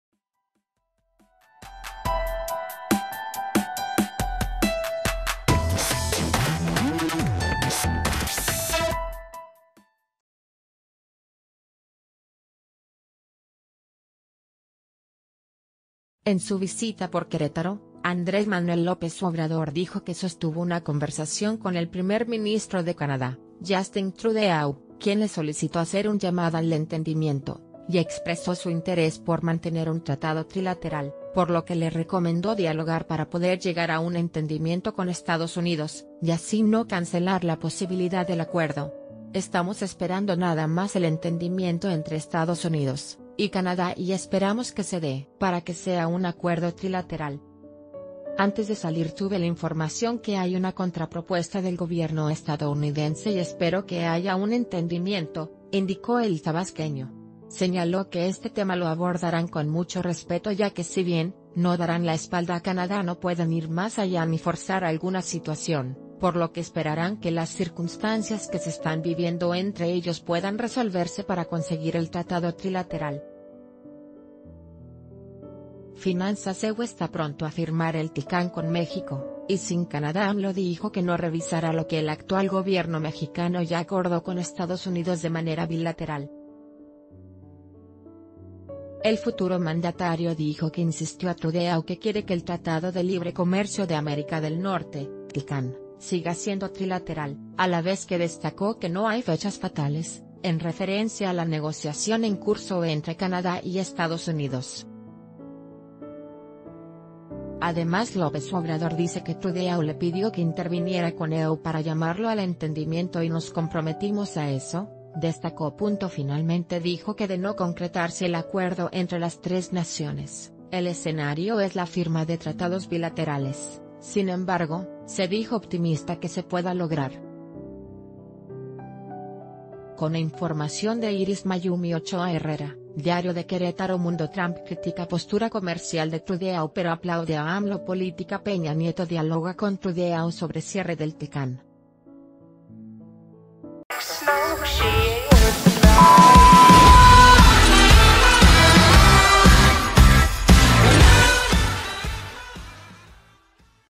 TLCAN debe ser trilateral, dice AMLO a Trudeau
En su visita por Querétaro, Andrés Manuel López Obrador dijo que sostuvo una conversación con el primer ministro de Canadá, Justin Trudeau, quien le solicitó hacer un llamado al entendimiento y expresó su interés por mantener un tratado trilateral, por lo que le recomendó dialogar para poder llegar ...